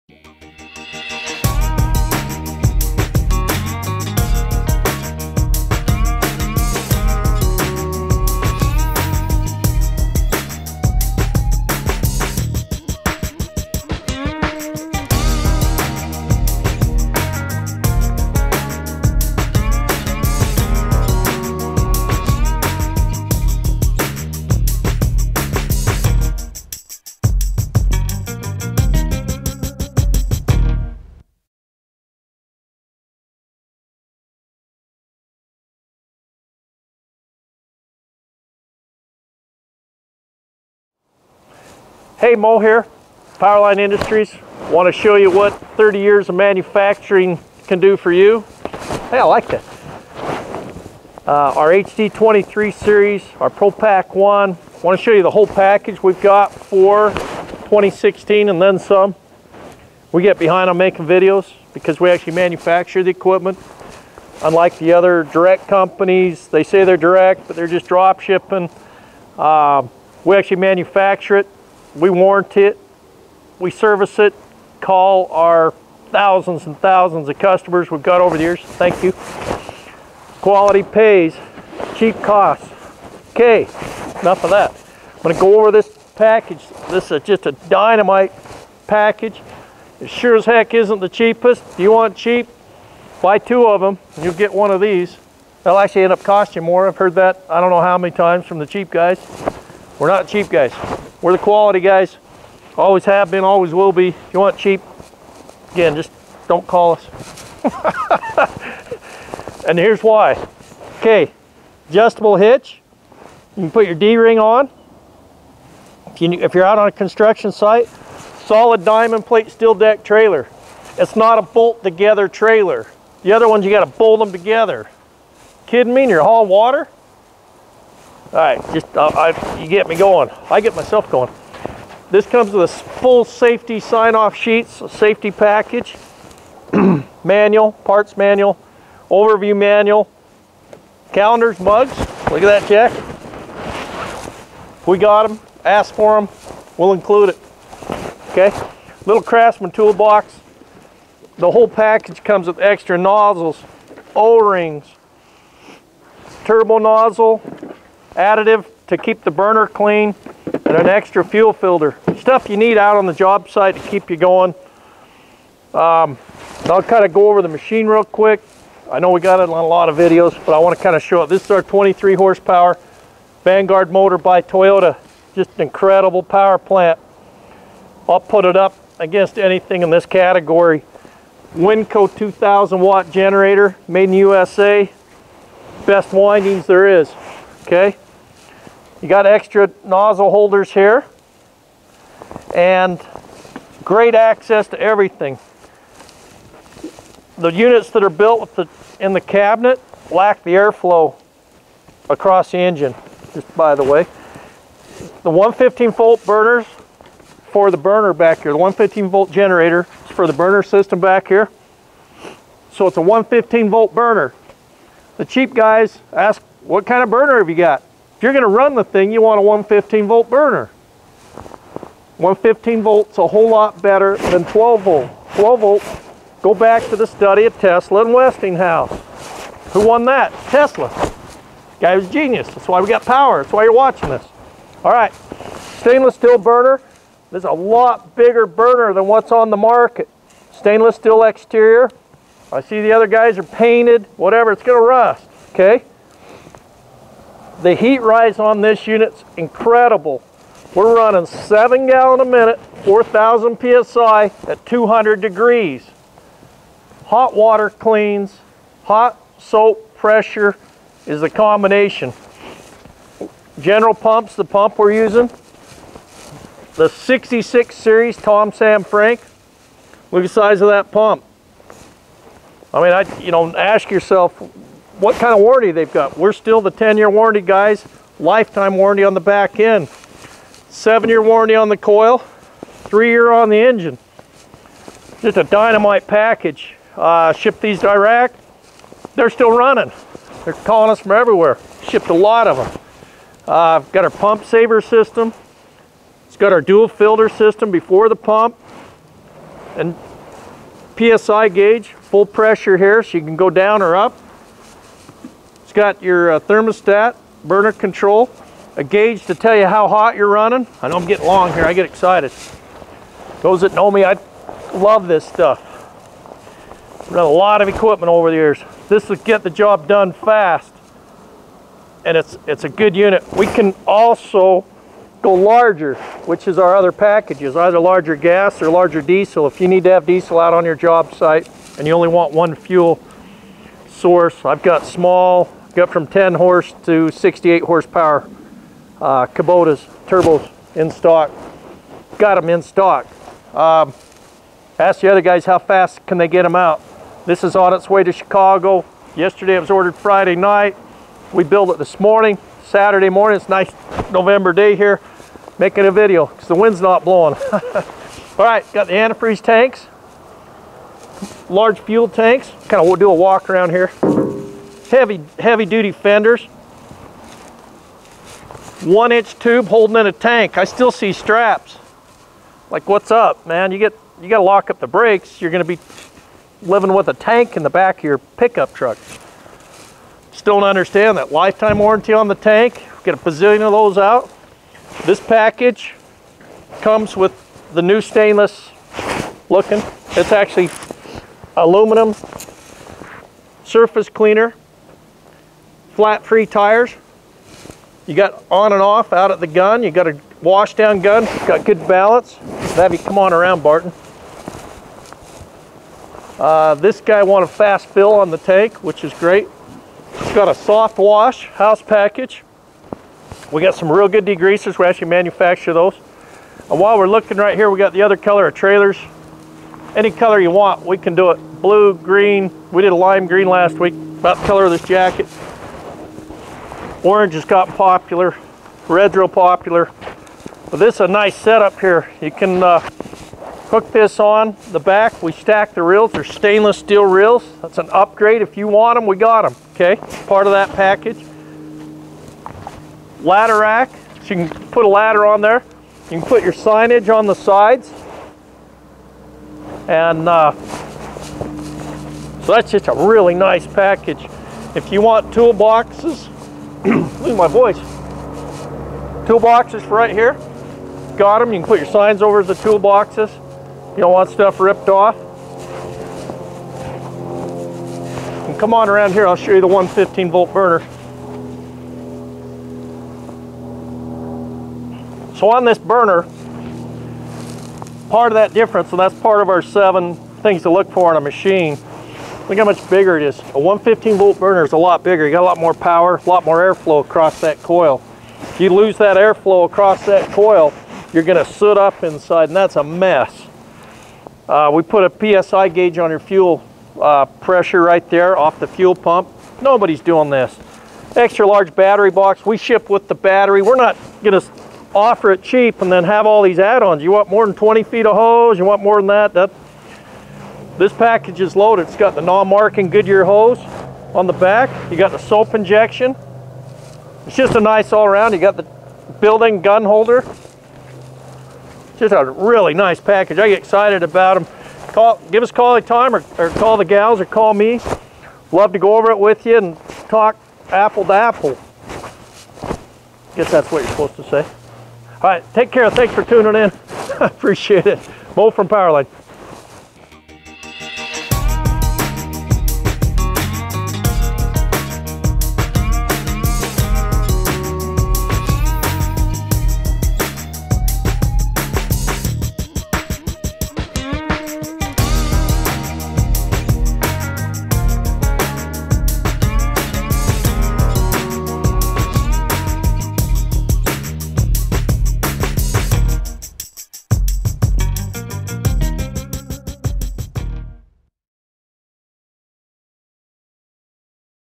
Power Washer Trailer Mounted Hot Water Industrial 23HP Monster Upgrade Package
The new Monster Upgrade Package Power Wash Trailer Direct industrial duty tandem axle pressure wash trailer system is engineered for the professional. This power wash trailer system features a high output of 7GPM @ 4,000PSi and is backed by the industry's best warranty. The HD23 features an extra large burner and guarantees super hot cleaning water.